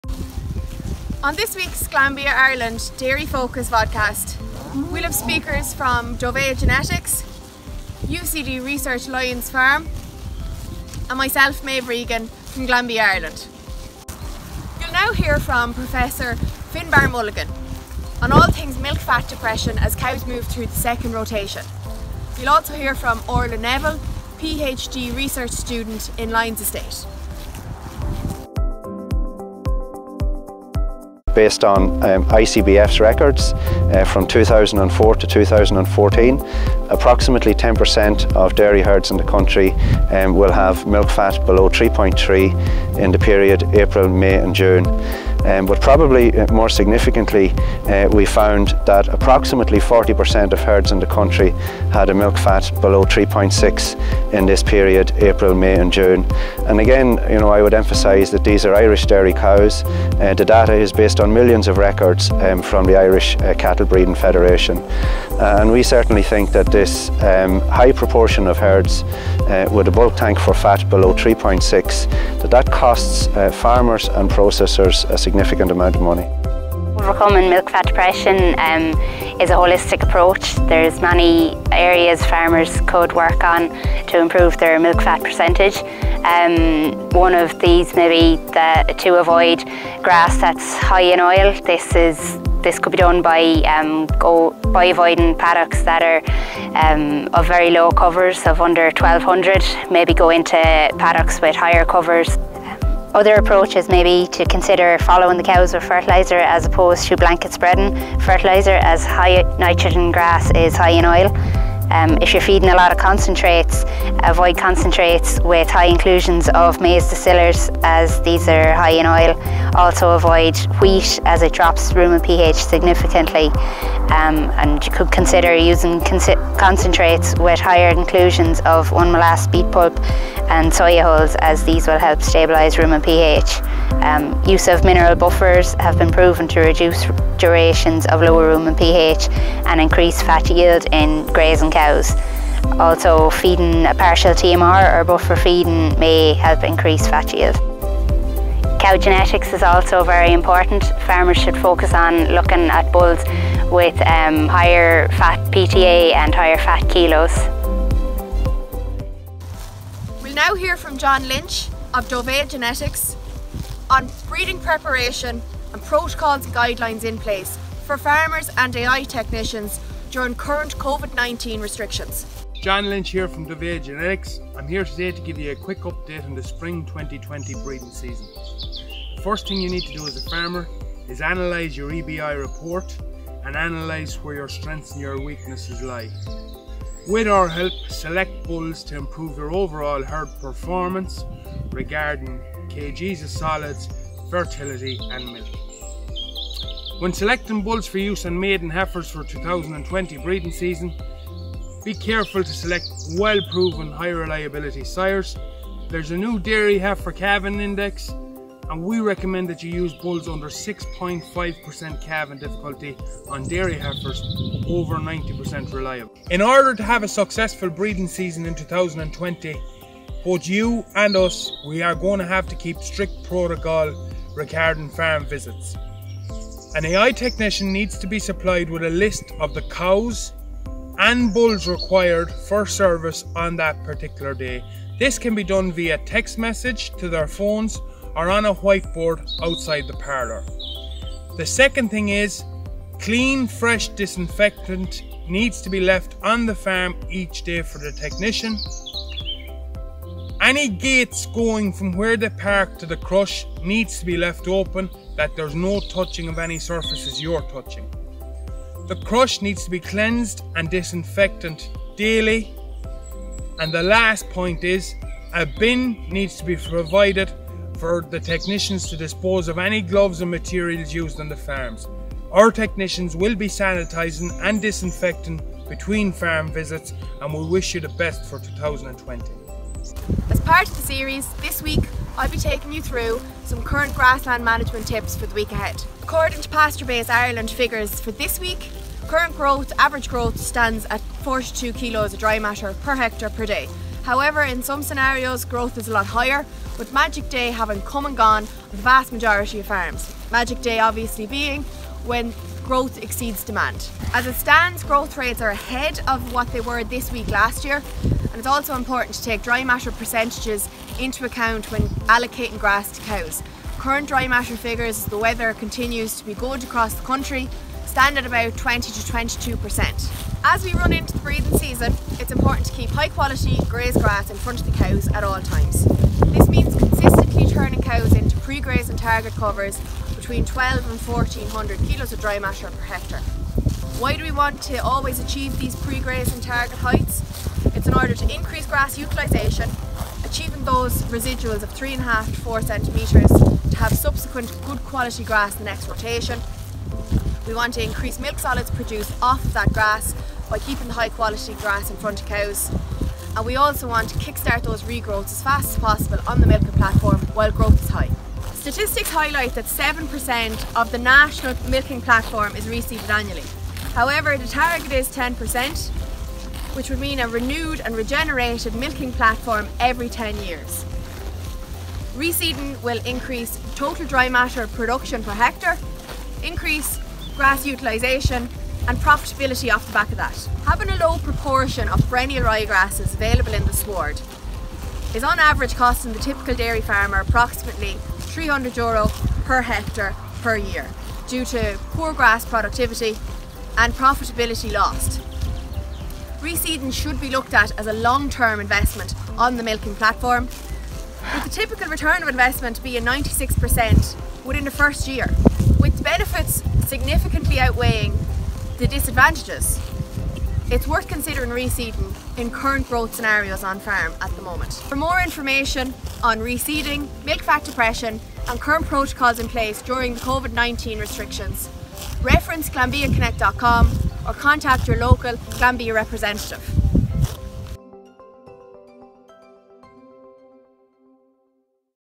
Dairy Focus Vodcast Episode 2
Watch the second episode of our DairyFocus Vodcast.

Finbar Mulligan and Orla Neville from UCD Lyons farm discuss the effect of Milk Fat Depression.

John Lynch from Dovea Genetics talks about selecting bulls for the coming breeding season.

Maeve Regan, Glanbia Ireland, gives an update on grassland management and reseeding.